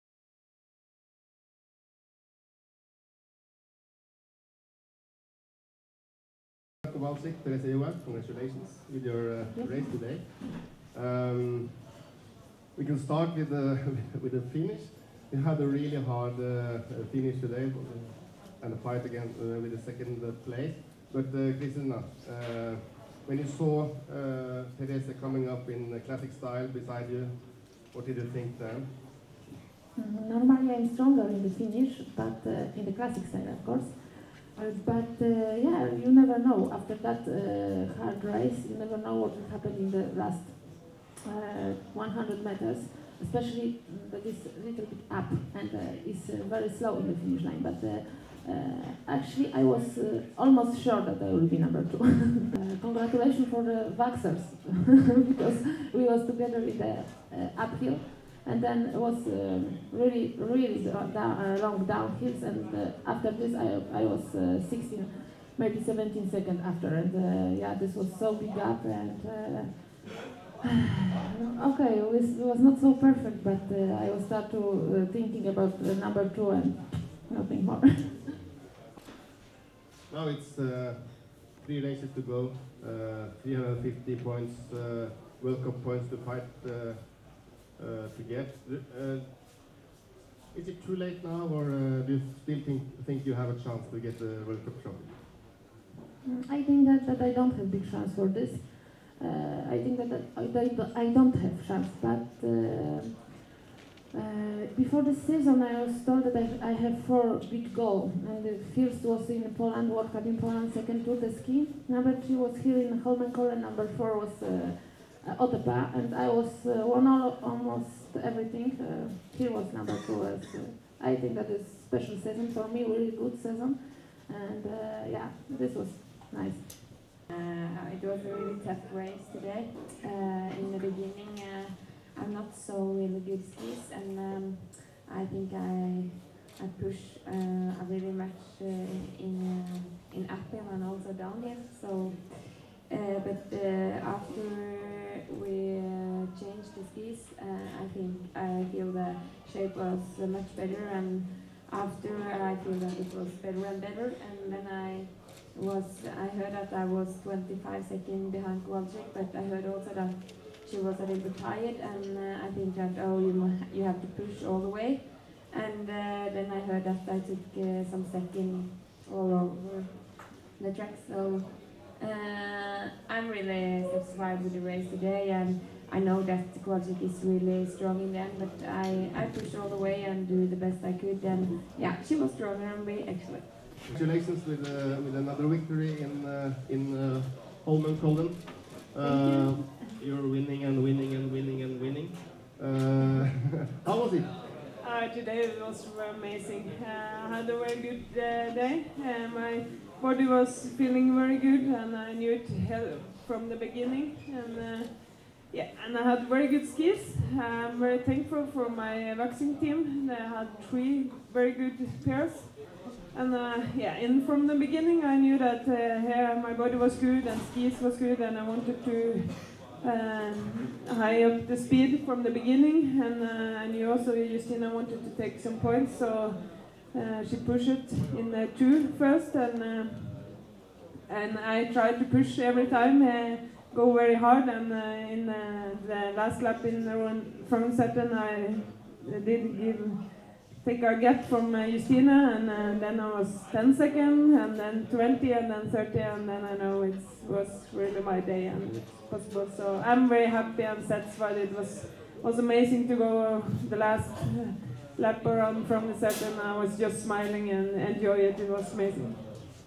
30 km Mass Start Classic
Marit Bjoergen (NOR), Justyna Kowalczyk (POL) and Therese Johaug (NOR) share their thoughts after 30 km Mass Start Classic at Holmenkollen